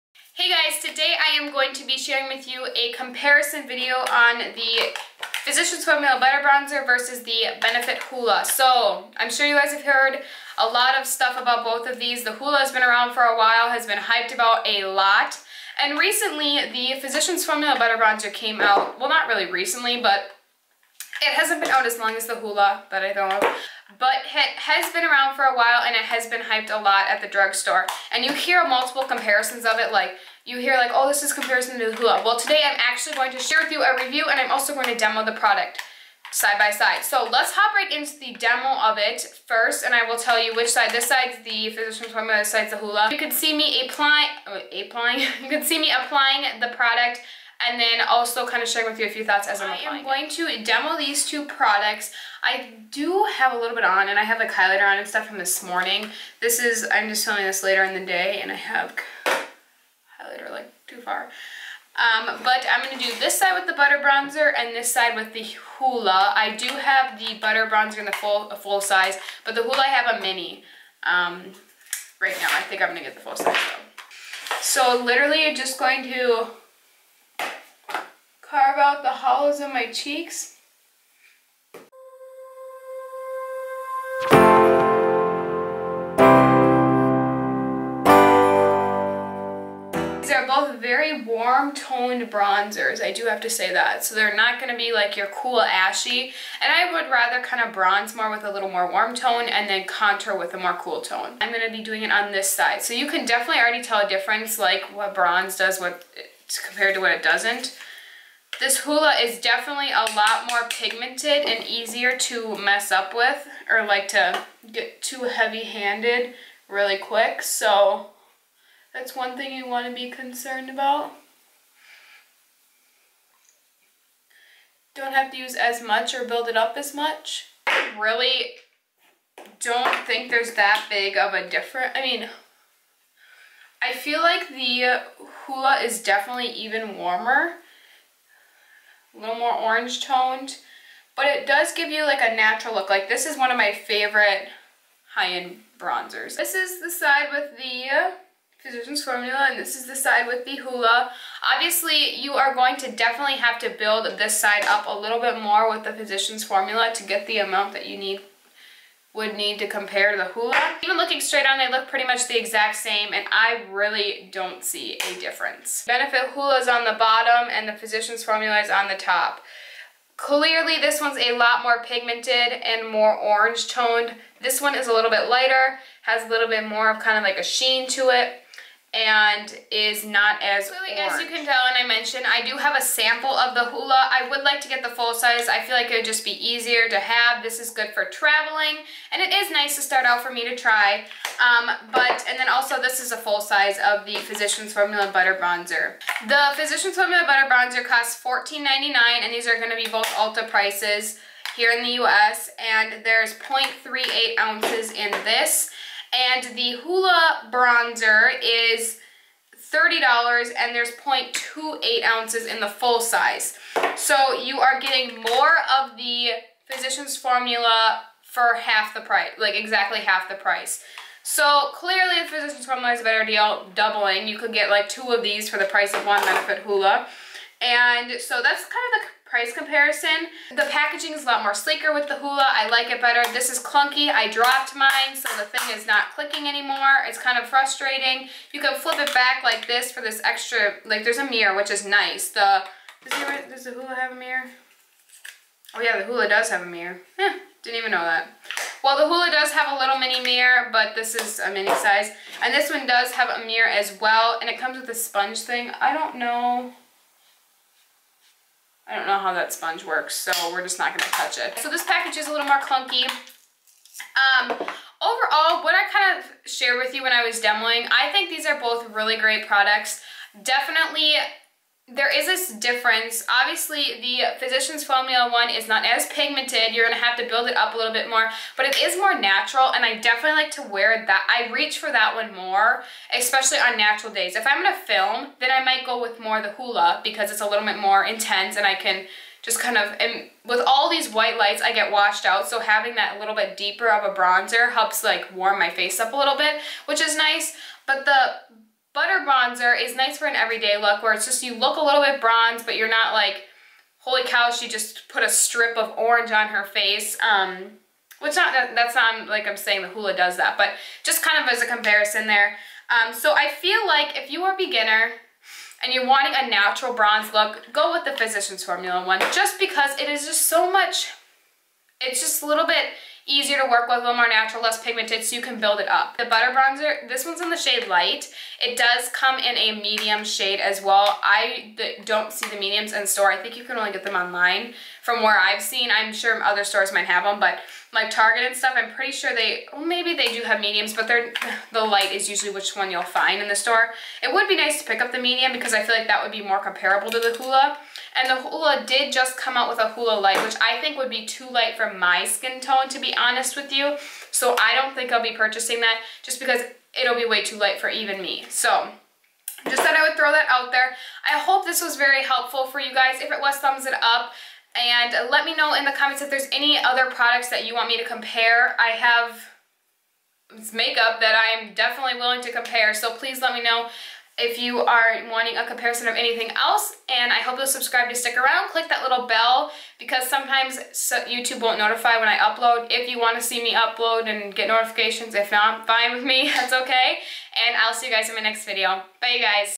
Benefit HOOLA vs Physicians Formula BUTTER BRONZER | Review & Demo
❋ SOCIAL MEDIA HANDLE: @simplyhope12

*Video(s) mentioned are linked in the 'i' at the top right corner of this video.

▼ PRODUCTS MENTIONED ▼
Benefit Hoola Bronzer
Physicians Formula Butter Bronzer (light)

Camera Used
Editing Software: Cyberlink Power Director